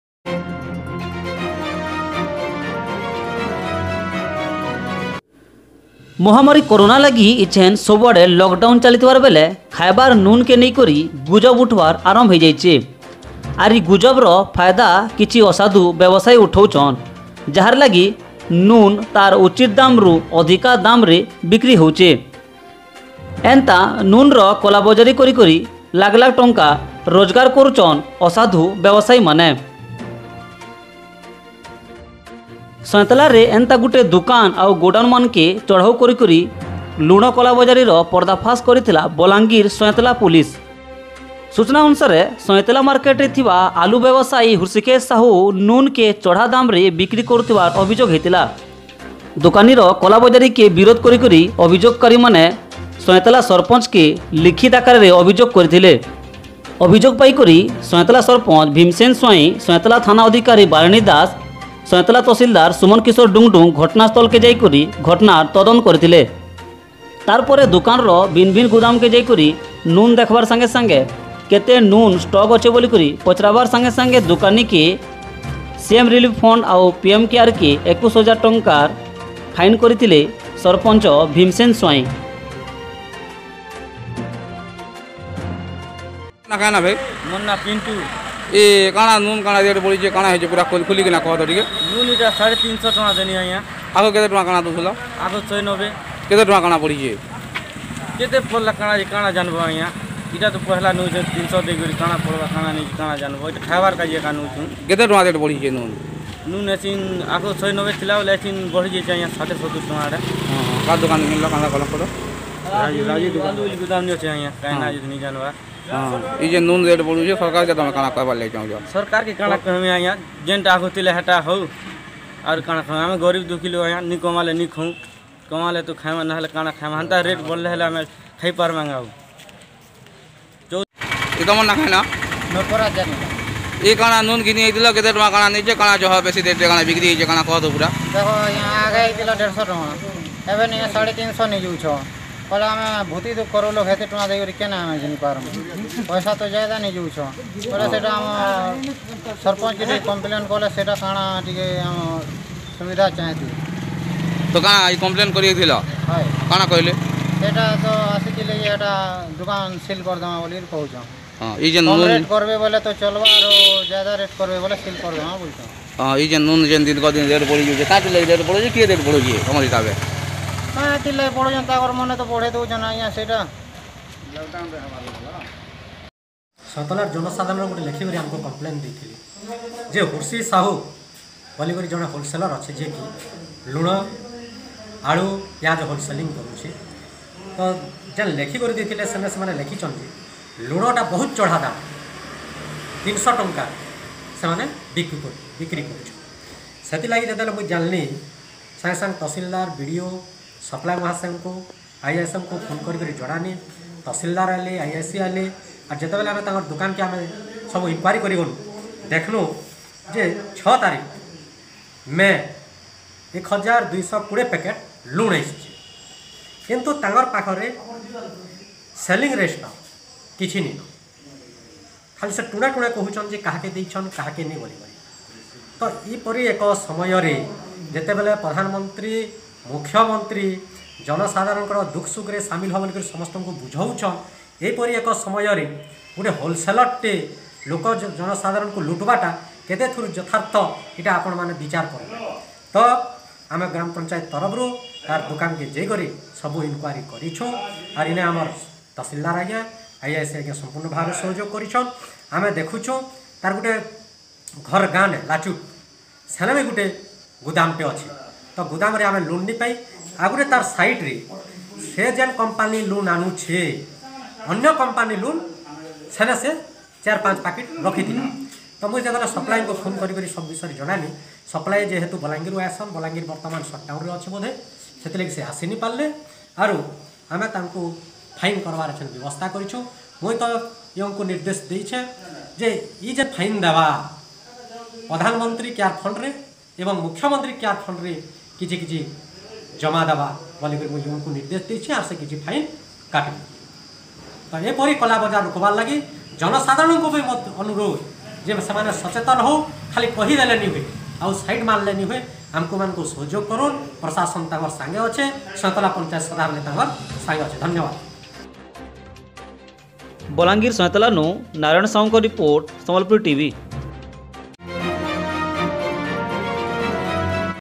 ଚଢାଦାମ ରେ ନୁନ୍ କେ ବିକ୍ରି କରି କଳାବଜାରୀ କରୁଥିବା ଦୁକାନ ଉପରେ ଚଢ଼ଉ, ୨୧ ହଜାର ଫାଇନ
SAMBALPURI TV